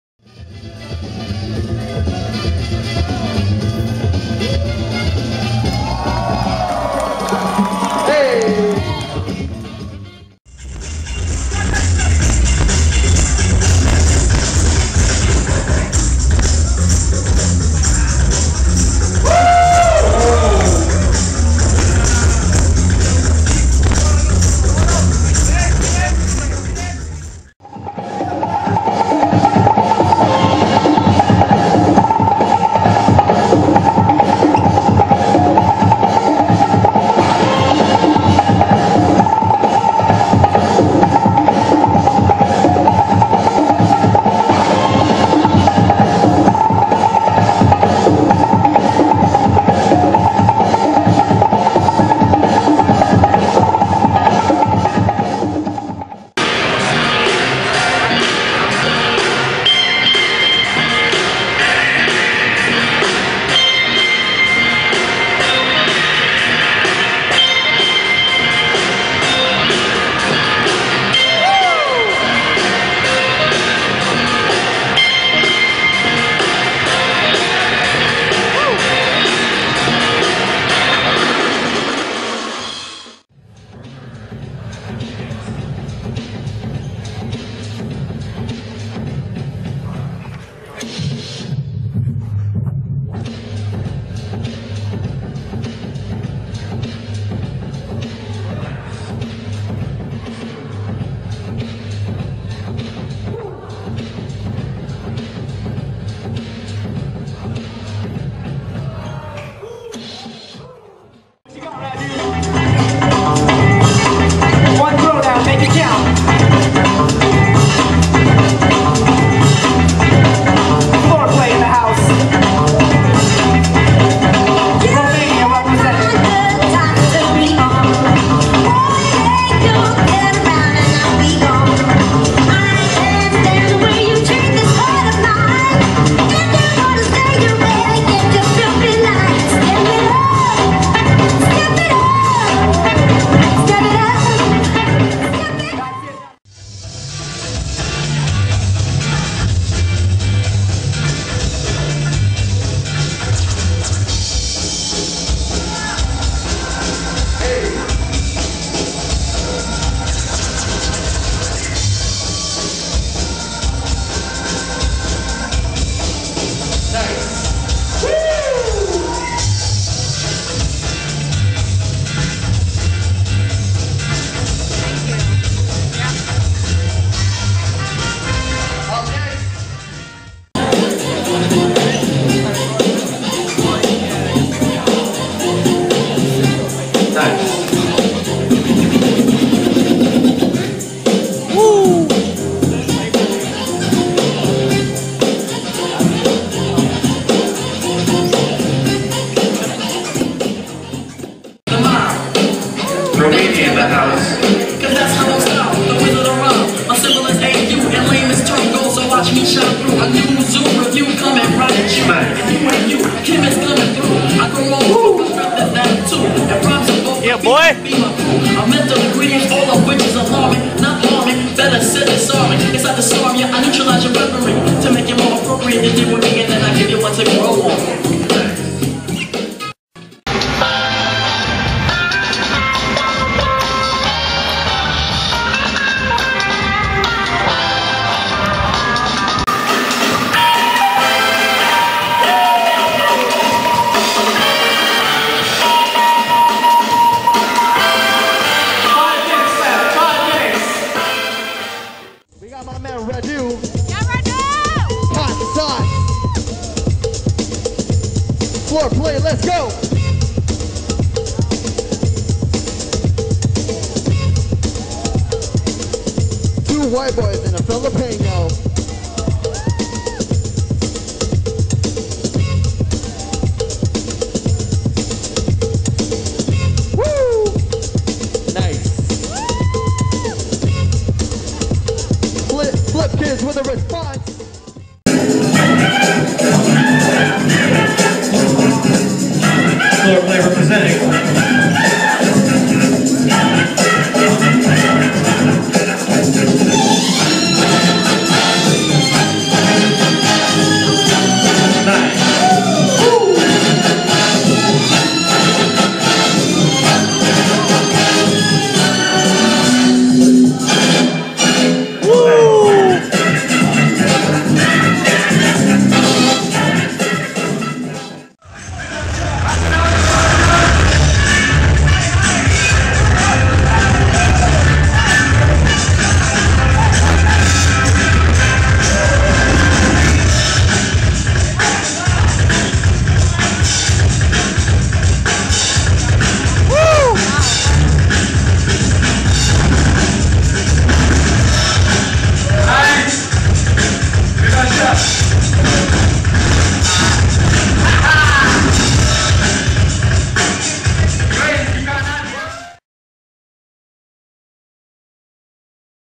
Radu - Freestyle Fanatic
a mix of freestyle runs from various competitions